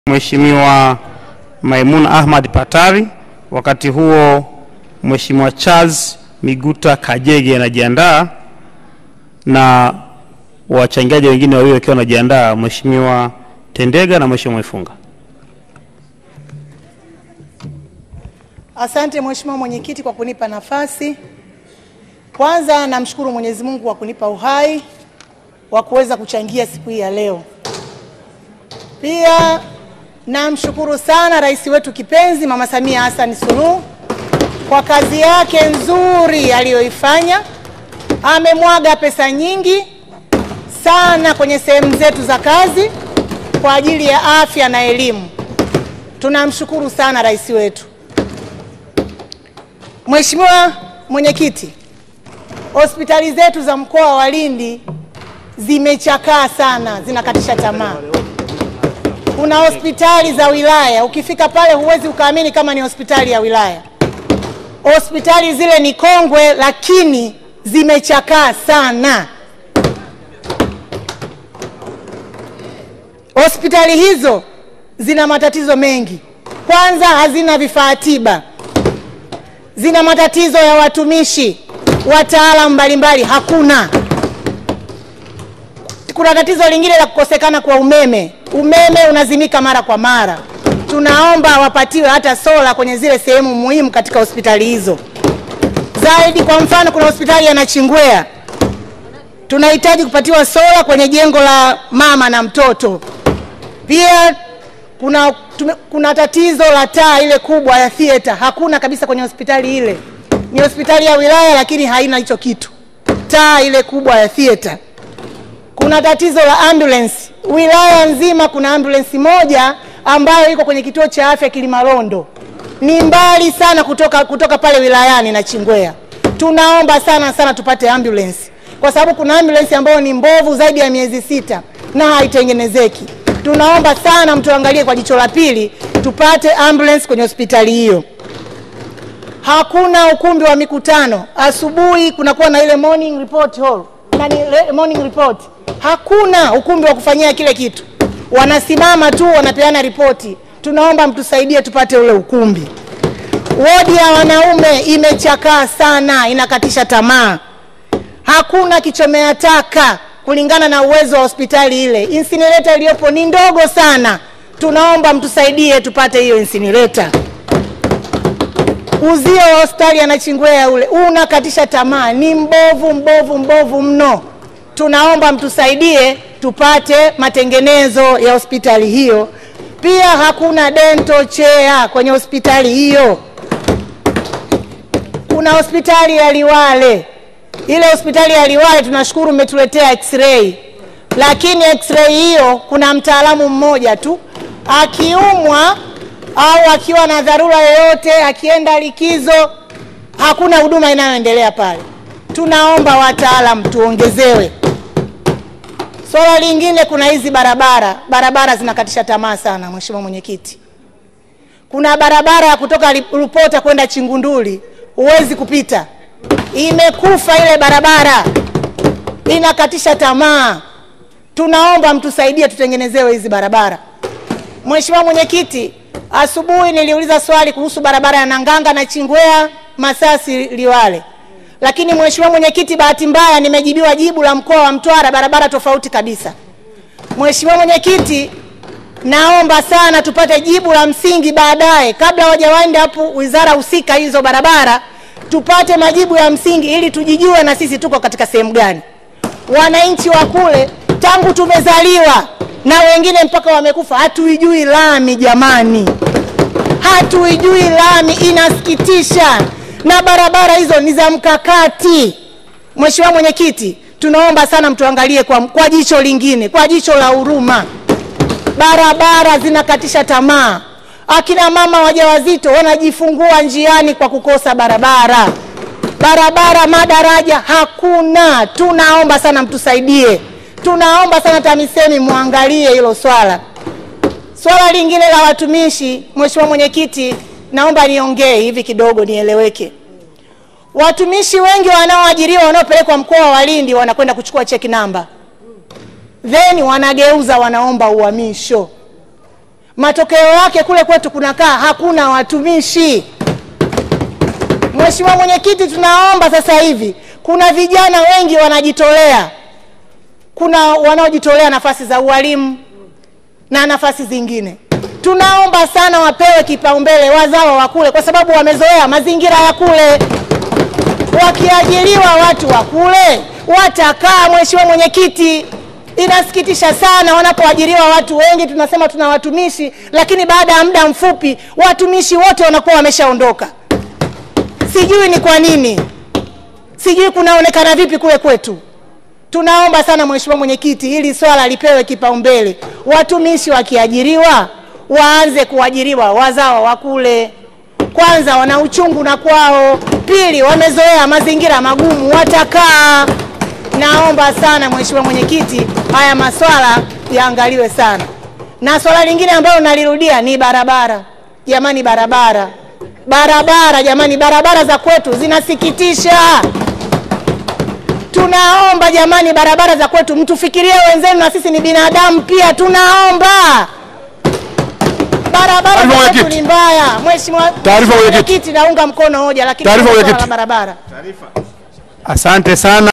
Mhe: Mbunge Viti Maalumu Akilia na Barabara Pamoja na maendeleo ya Wilaya yetu Nachingwea
Video from Shakir